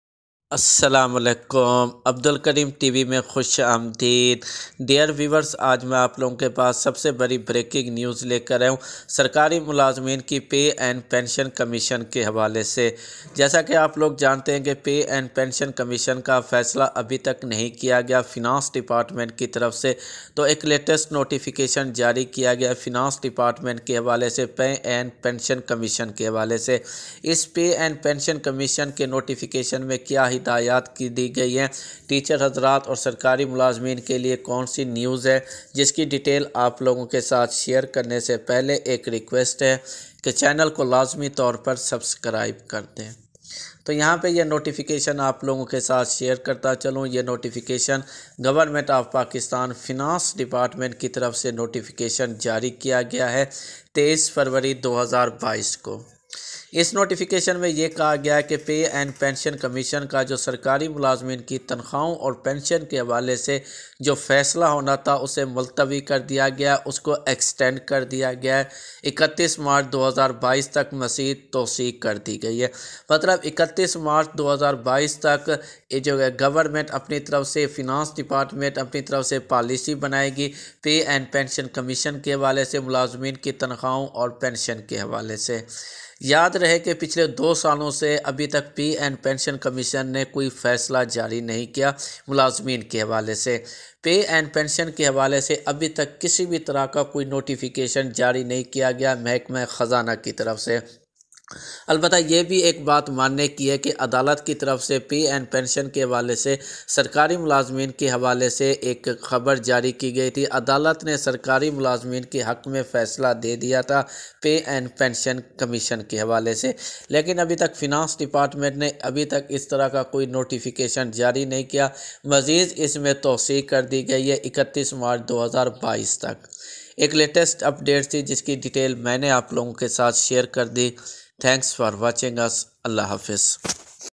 Pay & pension||Notification about pay and pension||Abdul karim
Welcome Abdul Karim Channel

Punjab education department issue new notification about the

Latest new notification about the pay and pension commission case i will share this news with you